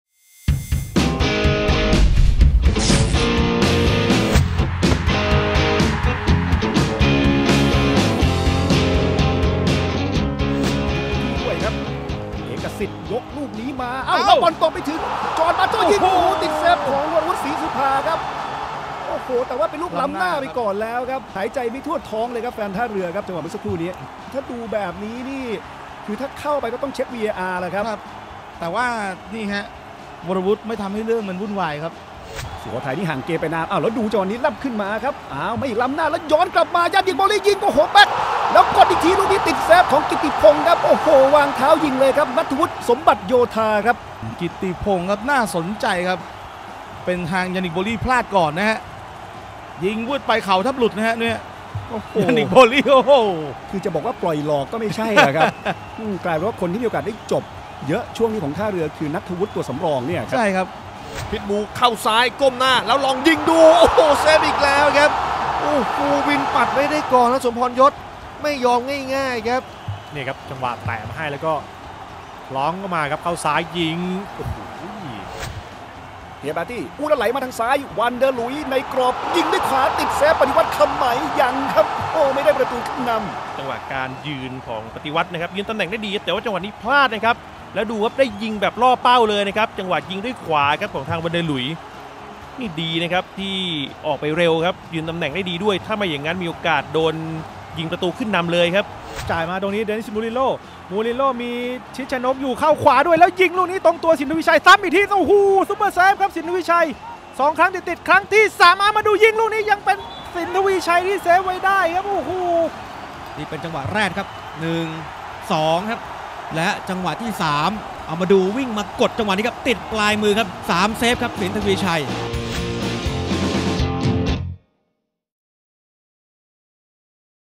Saves of the Week : สัปดาห์ที่ 17 | โตโยต้า ไทยลีก 2020
ย้อนชมการเซฟสุดสวยของผู้รักษาประตูใน โตโยต้าไทยลีก สัปดาห์ที่ 17 ที่ผ่านมา มาดูกันว่าจะมีลูกไหนบ้าง ?